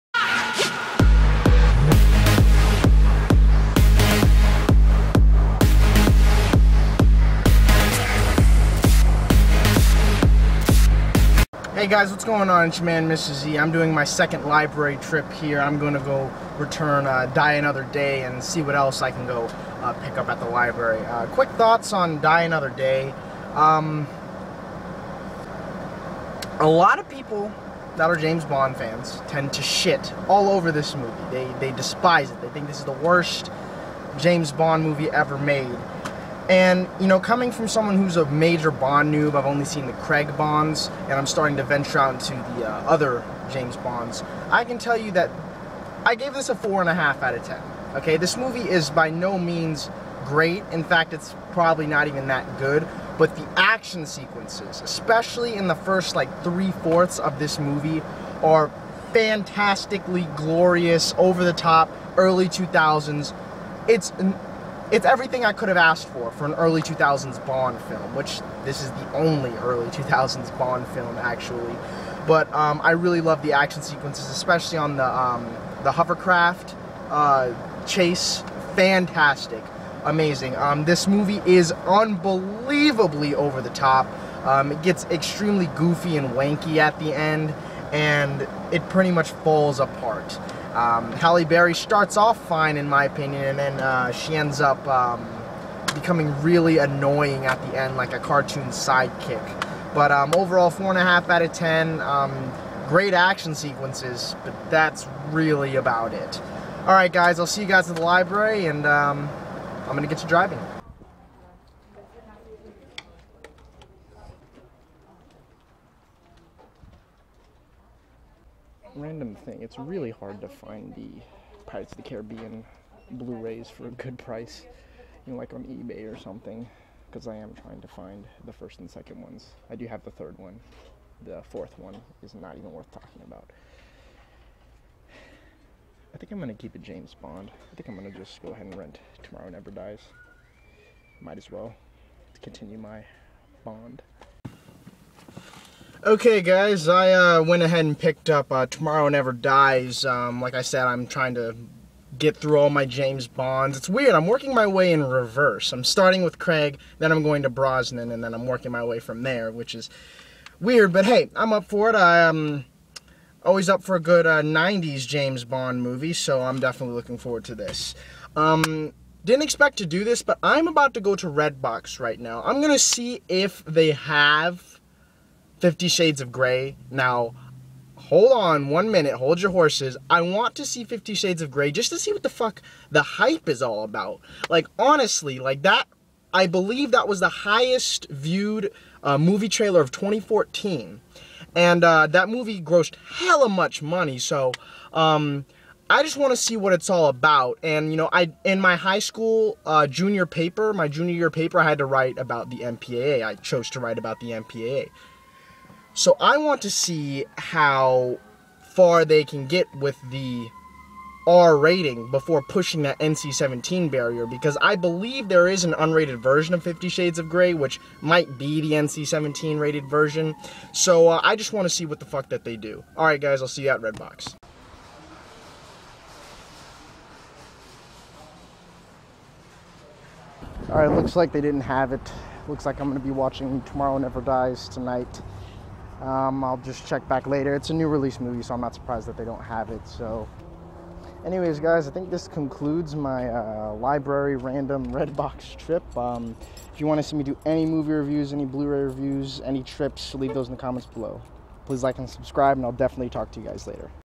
Library Movie Run #2
My 2nd run to my local library to pick up some movies.